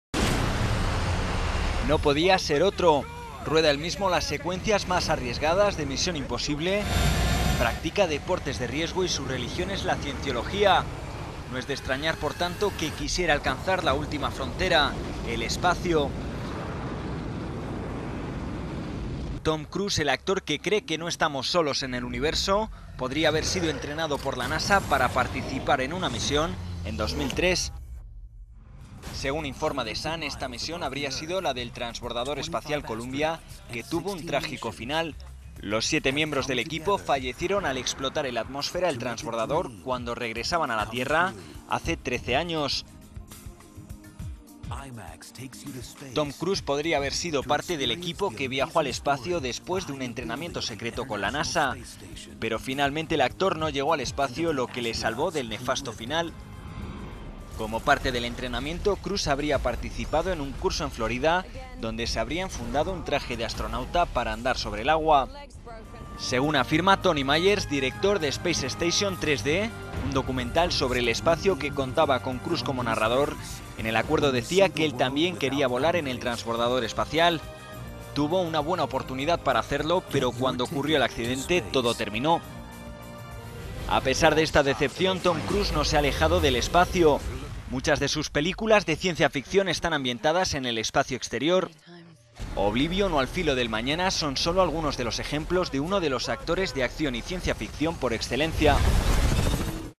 Tom Cruise entrenó en secreto con la NASA
No podía ser otro. Rueda él mismo las secuencias más arriesgadas de Misión Imposible, practica deportes de riesgo y su religión es la cienciología. No es de extrañar, por tanto, que quisiera alcanzar la última frontera: el espacio. Tom Cruise, el actor que cree que no estamos solos en el universo, podría haber sido entrenado por la NASA para participar en una misión en 2003.